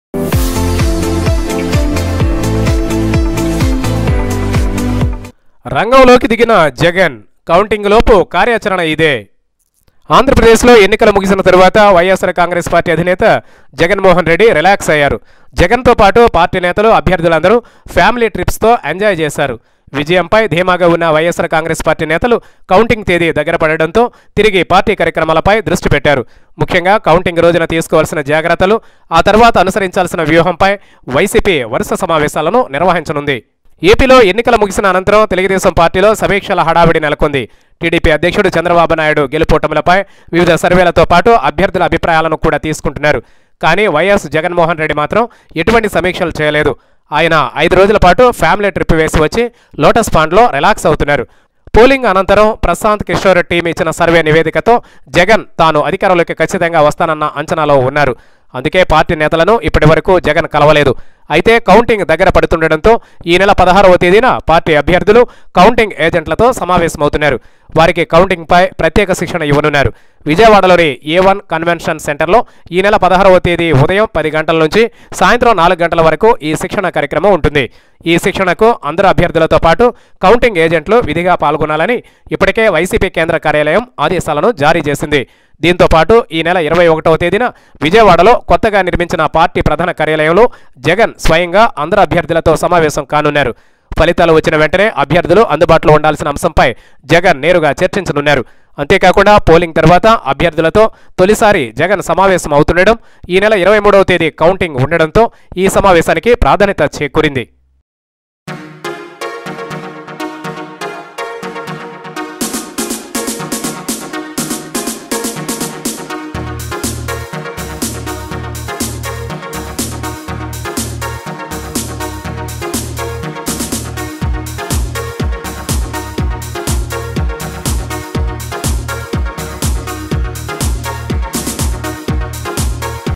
రంగంలోకి దిగిన జగన్ కౌంటింగ్ లోపు యాక్షన్ ప్లాన్ ఇదే| YS Jagan Action Plan before election results
YSR Congress Chief YS Jagan has been activated after the election. Here is the YS Jagan's action plan before the election counting.